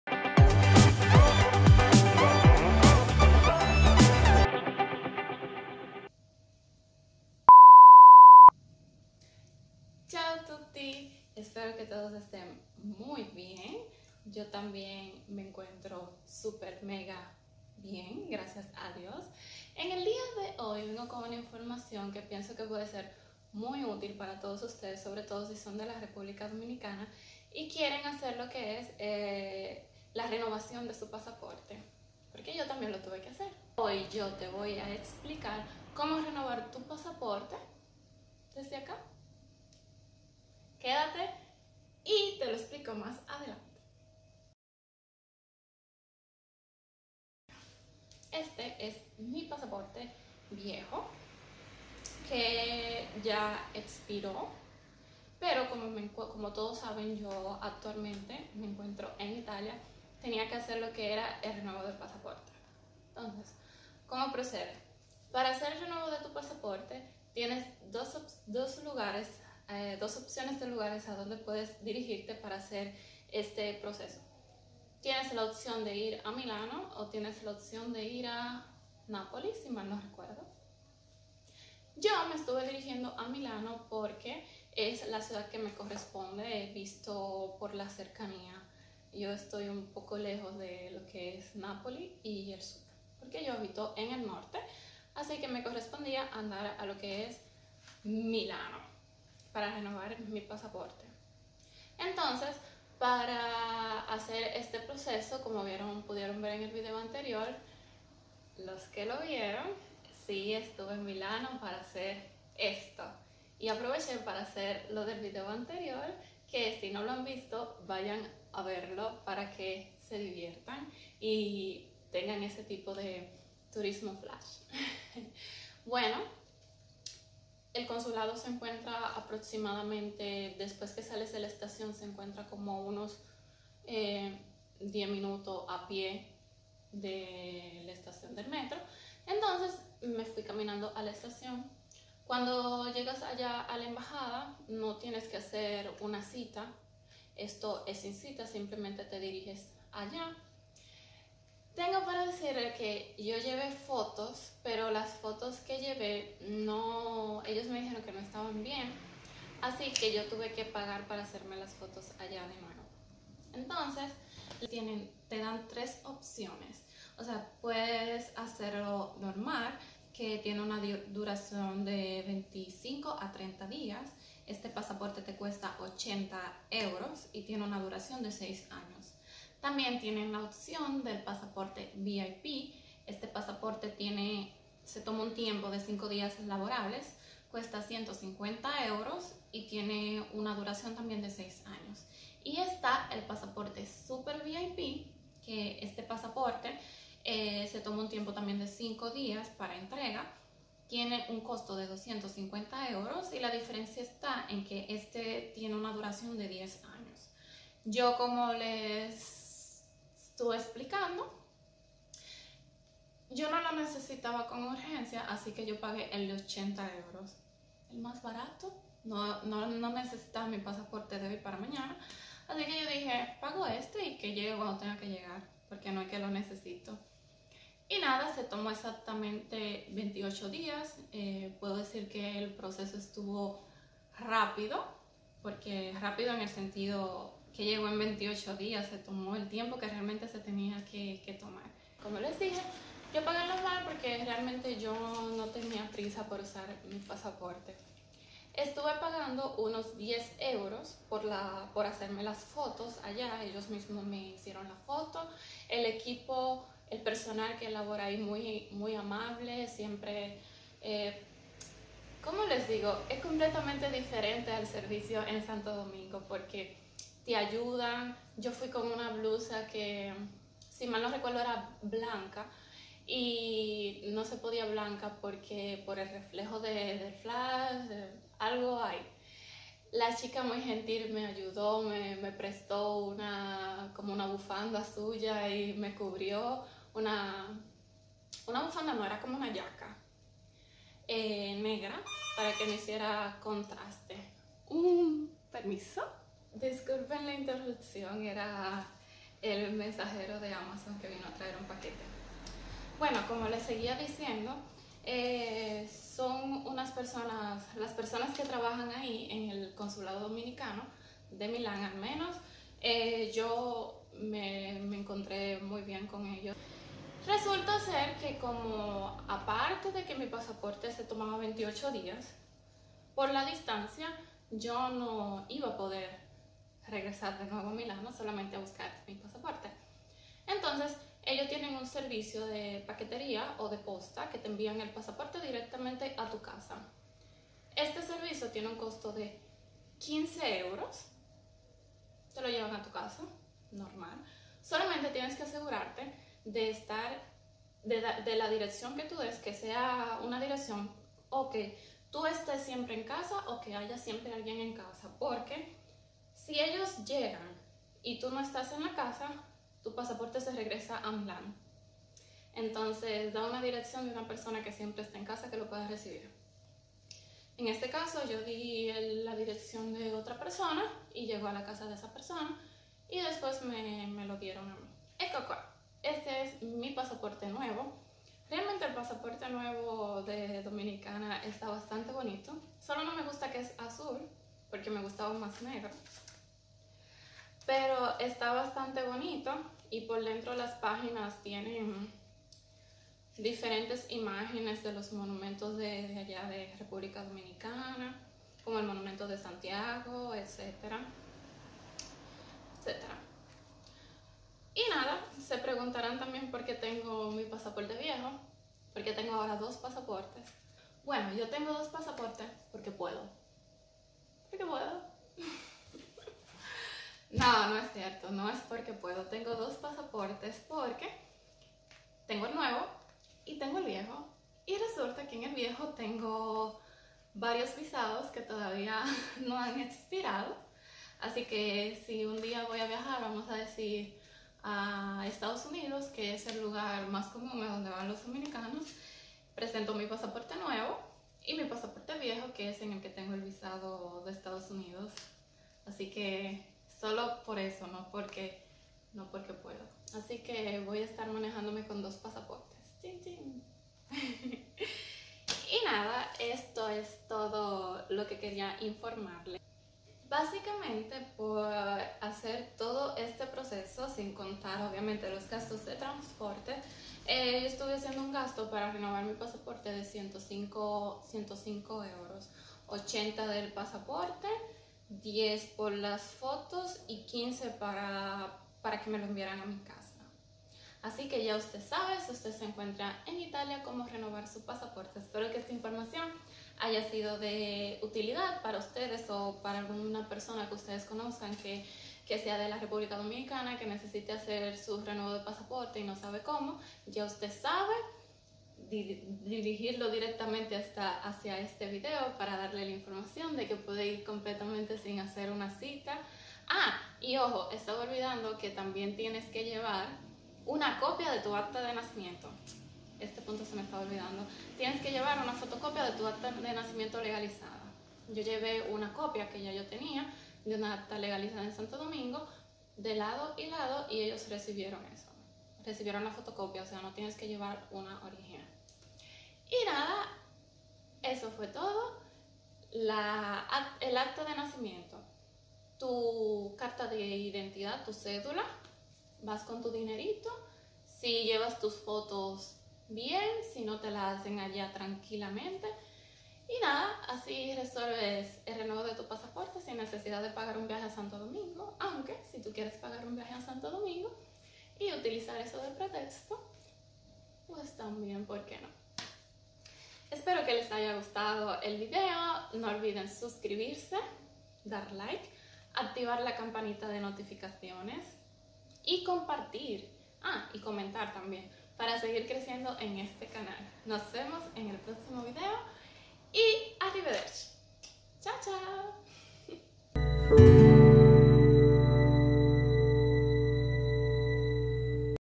COMO RENOVAR TU PASAPORTE SI ESTAS EN ITALIA... AQUI TE LO EXPLICO TODO
Ciao tutti, espero que todos esten muy bien... La informacion que les comparto en el dia de hoy espero que sea de utilidad para todos ustedes o algun conocido si se encuentran en Italia y necesitan renovar su pasaporte pero no saben como hacer, pues aca les digo detalladamente como y donde tienen que dirigirse , como siempre no olviden suscribirse, activar la campana de notificaciones, comentar, dar like y compartir, les mando un grande abrazo desde este lado del mundo y muchos besos. Arrivederci!